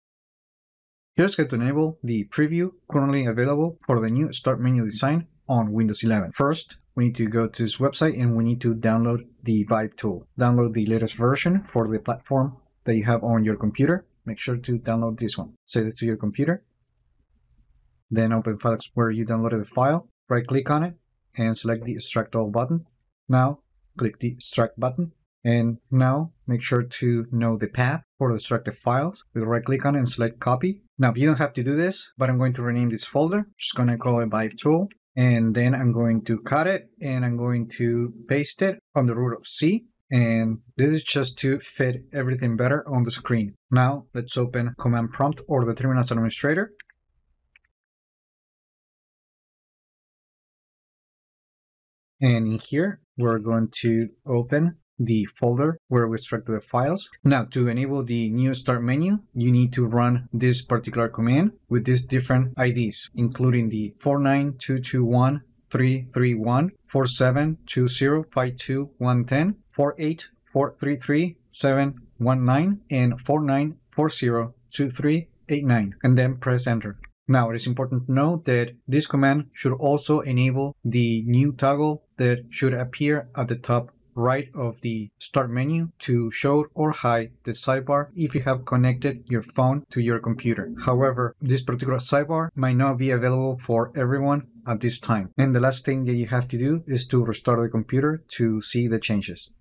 Enable new Start menu for Windows 11 2025 with ViveTool – Are you trying it?
You can enable the new Start menu coming to Windows 11, which includes a larger layout and the ability to disable the recommended section using the ViveTool.